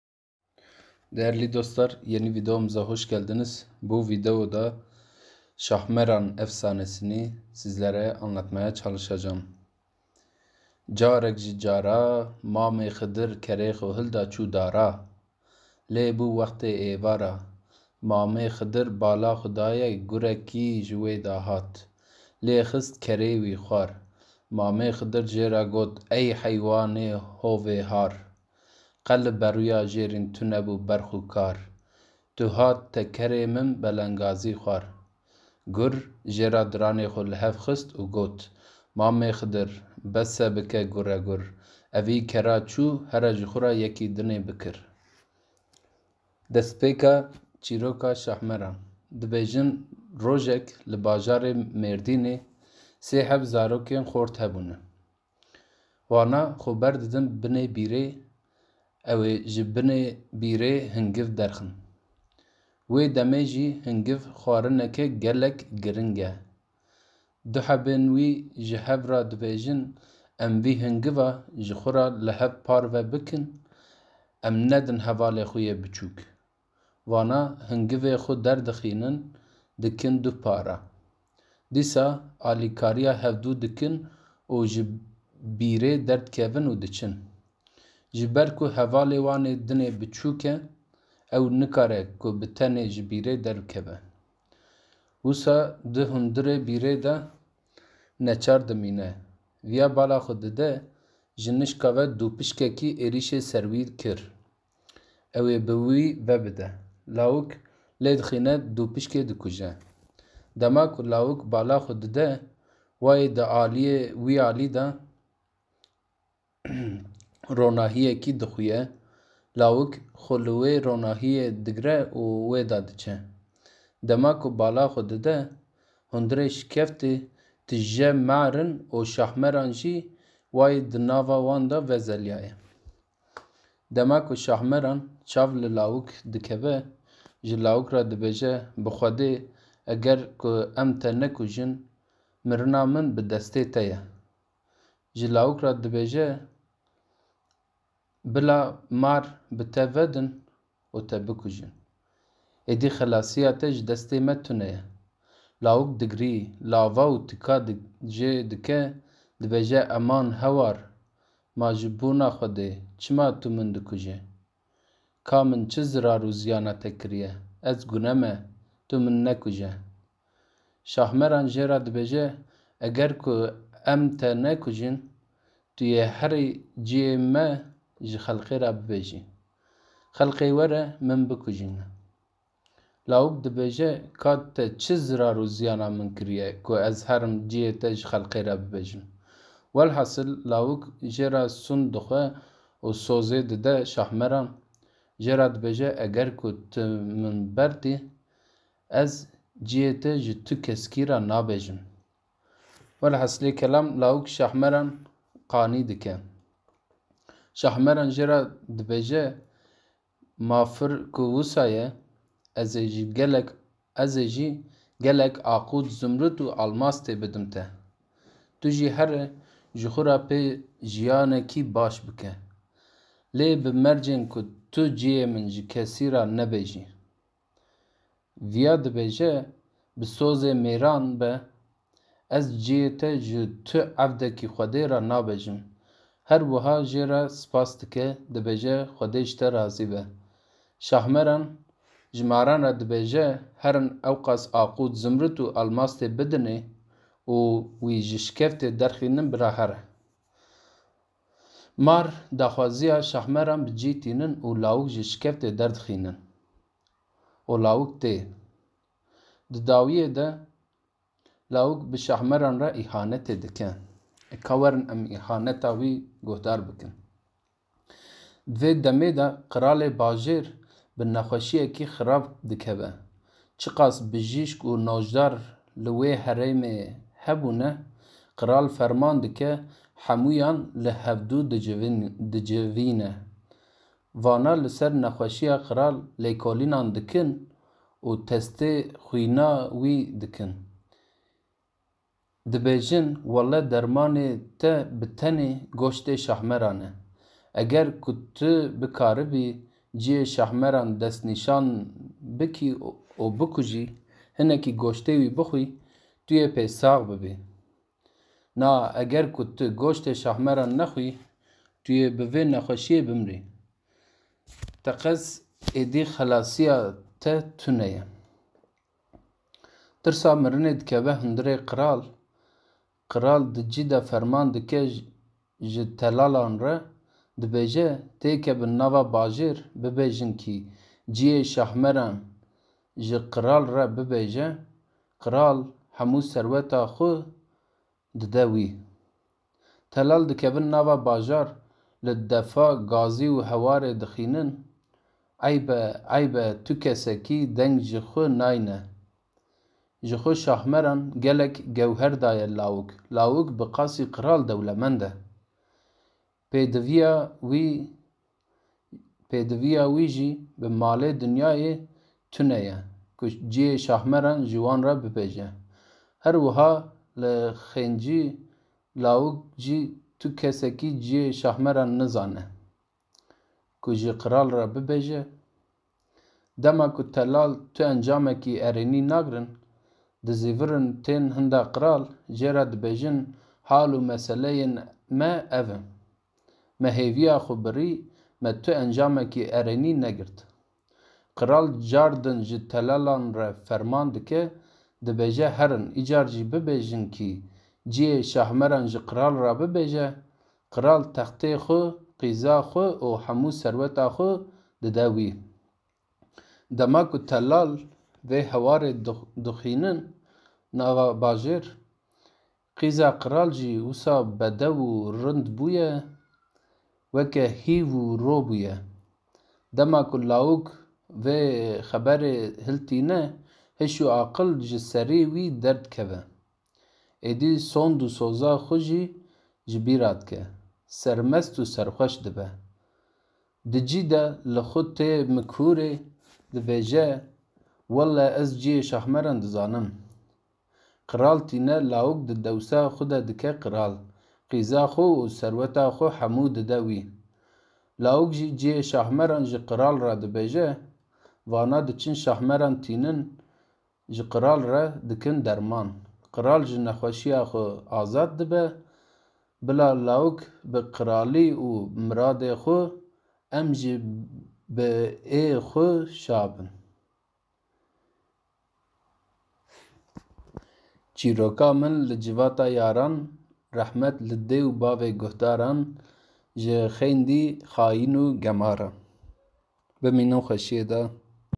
ŞAHÊ MERAN KÛRDİ
DEĞERLİ DOSTLAR ÖNCELİKLE SÜRÇÜ LİSAN ETTİYSEK AFFOLA.MİTOLOJİK BİR VARLIK OLAN ŞAHMERAN EFSANESİNİ ANLATMAYA ÇALIŞTIK.İYİ DİNLEMELER